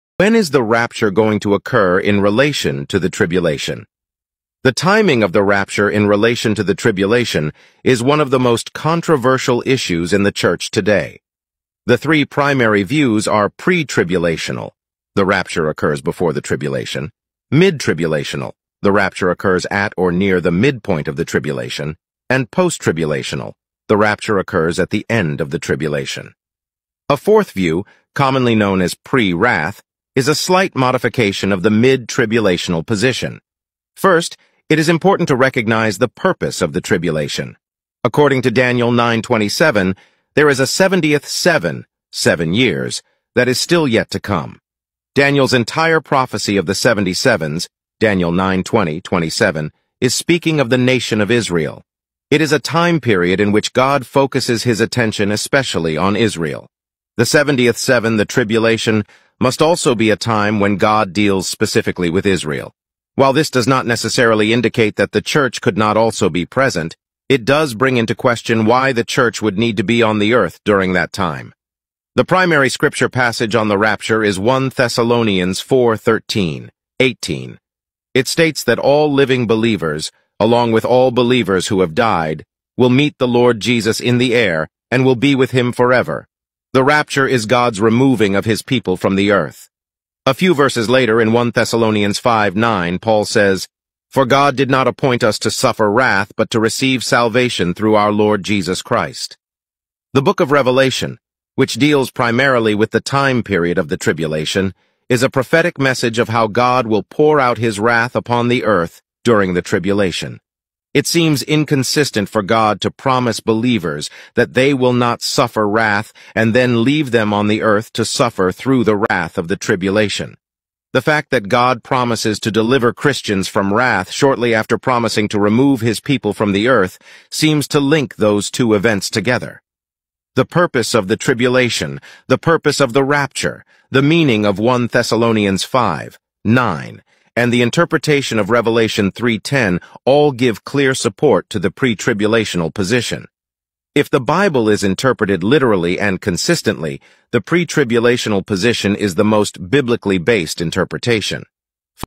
What's The Timing Of The Rapture?
**The Timing of the Rapture: Exploring Biblical Prophecy**

Welcome to our in-depth analysis on one of the most intriguing topics in Christian eschatology - the timing of the Rapture. Join us as we delve into various biblical interpretations, historical contexts, and theological perspectives that shape our understanding of this prophetic event.

In this video, we'll cover:

- What is the Rapture?
- Key Bible verses and their interpretations
- Different views on the timing: Pre-Tribulation, Mid-Tribulation, and Post-Tribulation
- Historical and contemporary perspectives
- What the Rapture means for believers today

Whether you're a seasoned scholar, a curious believer, or someone seeking answers, this video aims to provide a comprehensive overview of the Rapture's timing.